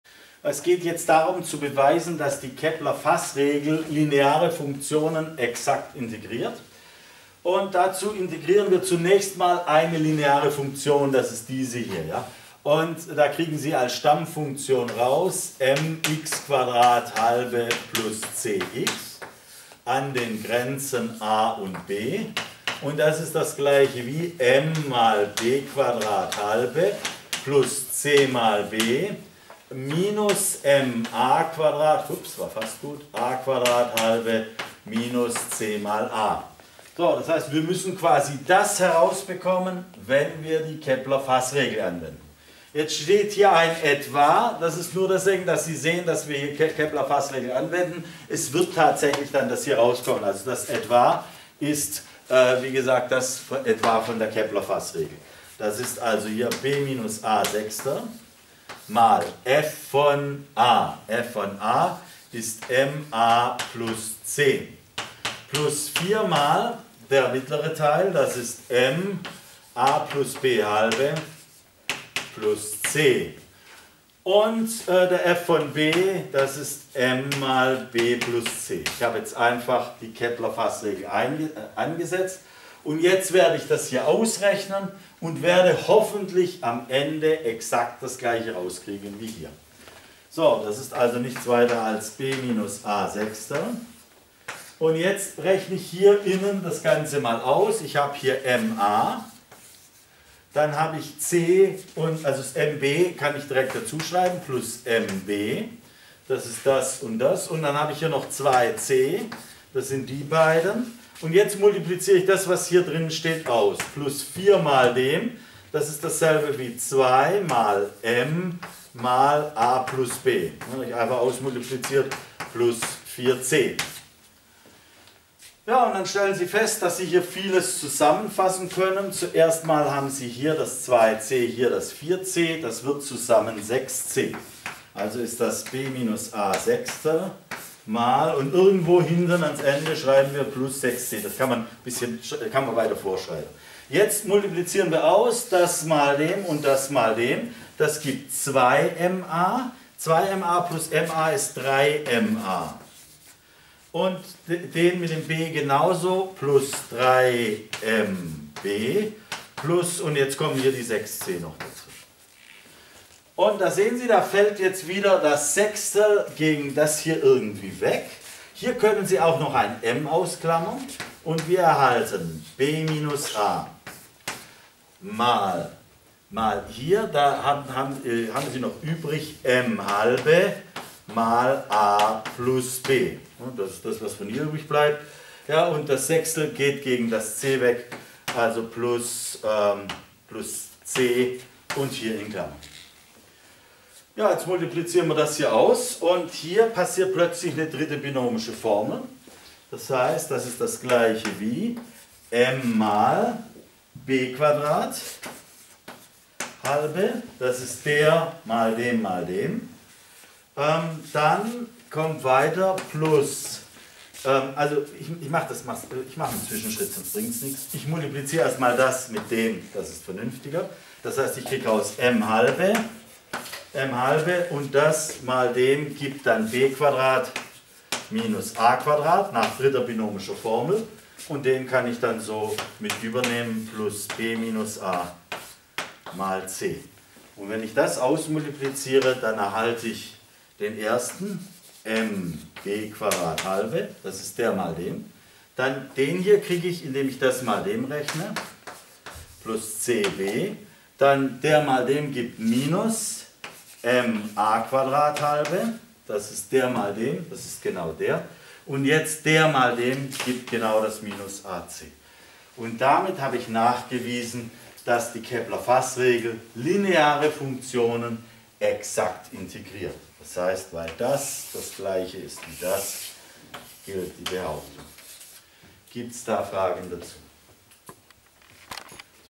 Keplersche Fassregel integriert lineare Funktionen exakt(183/495) | Mathematik vom Mathe Schmid
Aufgabe 183/495 aus Hurra Mathe (Version 7.1) gefilmt am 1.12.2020 Alle meine Projekte finden Sie
Weitere Infos finden Sie unter Sd.slt.biz und unter Buch.slt.biz